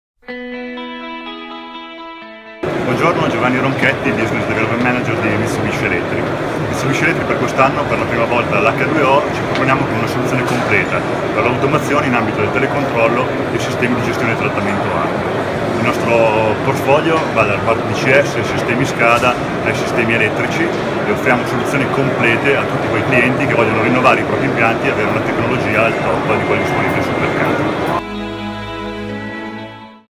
H2O Accadueo 2016 | Mitsubishi Electric Europe B.V. - Filiale Italiana
In occasione di ACCADUEO 2016, a BolognaFiere, dal 19 al 21 ottobre, Mitsubishi Electric Europe B.V., per la prima volta al Salone di Bologna, ha proposto soluzioni che vanno dalle automazioni nell’ambito del telecontrollo ai sistemi di gestione nel trattamento acque - video by Oplay